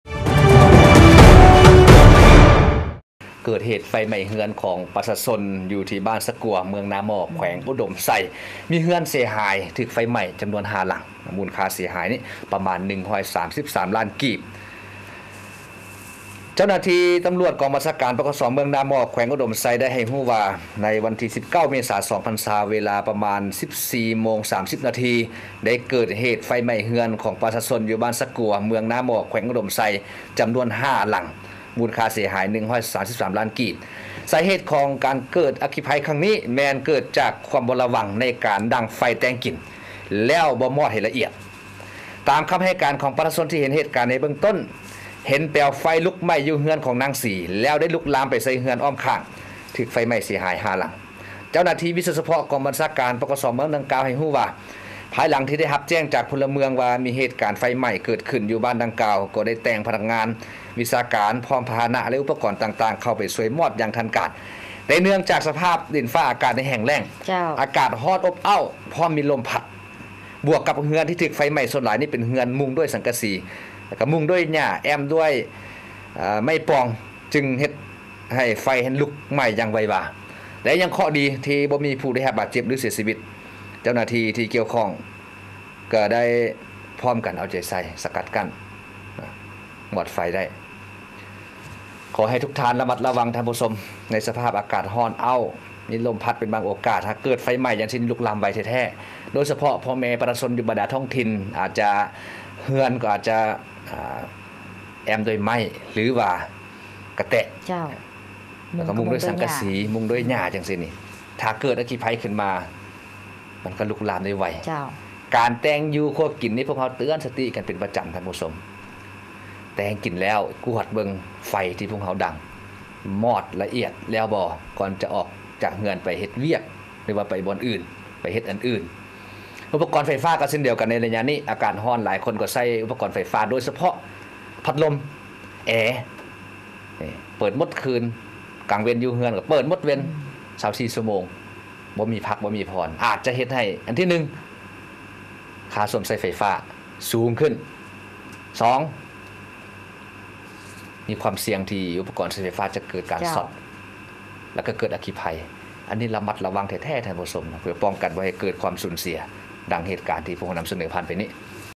ຂ່າວ ປກສ LaoPSTV news:24/04/2020 ເກີດເຫດໄຟໄໜ້ເຮືອນພໍ່-ແມ່ປະຊາຊົນເຂດບ້ານສະກົ່ວ ເມືອງນາໜອກ ແຂວງອຸດົມໄຊ
ສະຖານີ ໂທລະພາບ ປ້ອງກັນຄວາມສະຫງົບ  LAO PSTV 24/04/2020 ເກີດເຫດໄຟໄໜ້ເຮືອນພໍ່-ແມ່ປະຊາຊົນເຂດບ້ານສະກົ່ວ ເມືອງນາໜອກ ແຂວງອຸດົມໄຊ